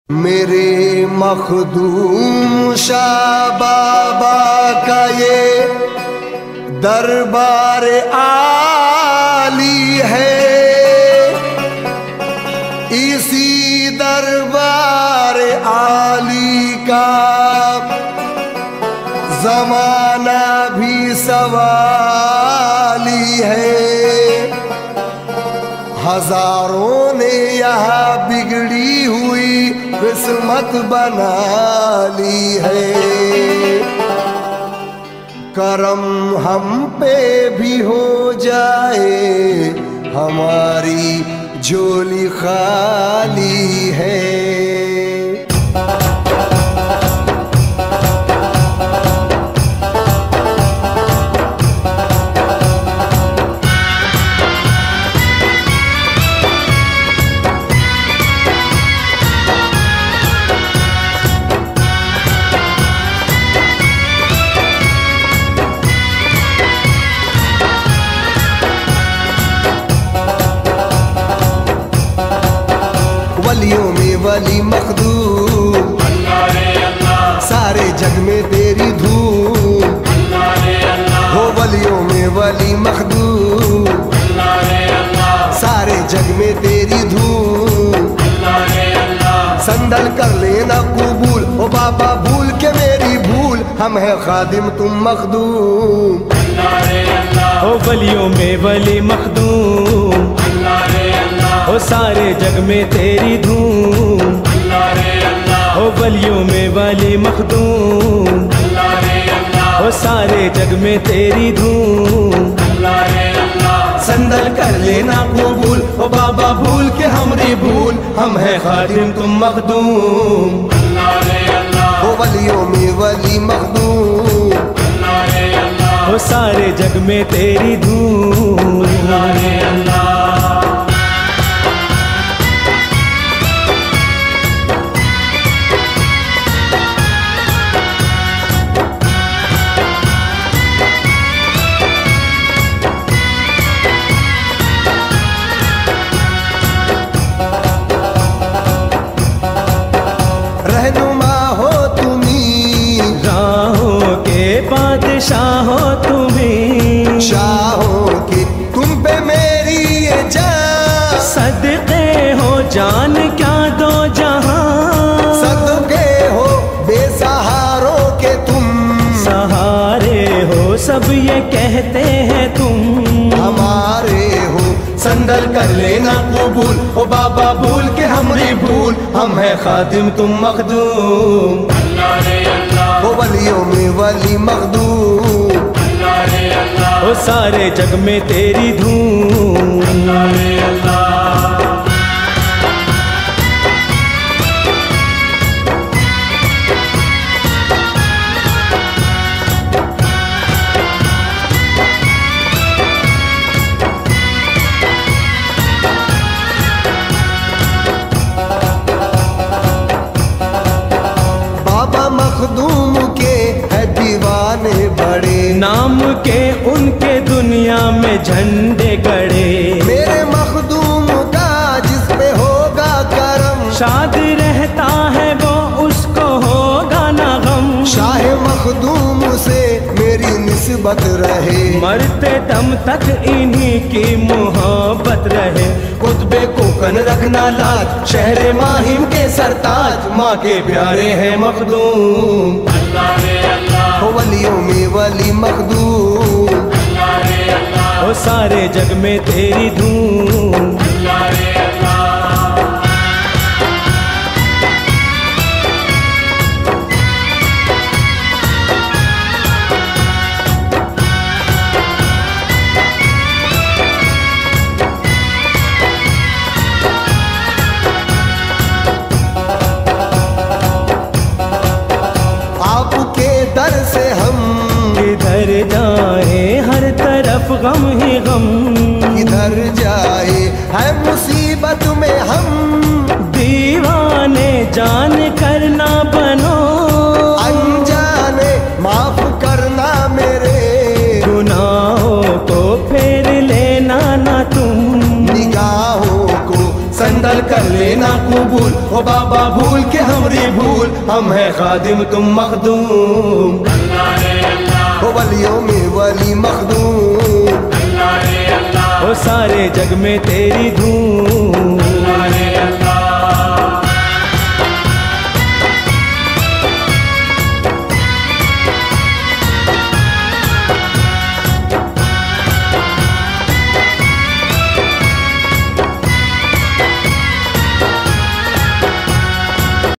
Shehr E Mahim Ke Sartaj / Urs Mubarak 607 / Ruksar Shaikh
Shehr E Mahim Ke Sartaj

Hazrat Makhdoom Ali Mahimi ◌ؒ Dargah Sharif
Inside Photos Of Urs Mubarak 607

Editor : Ruksar Shaikh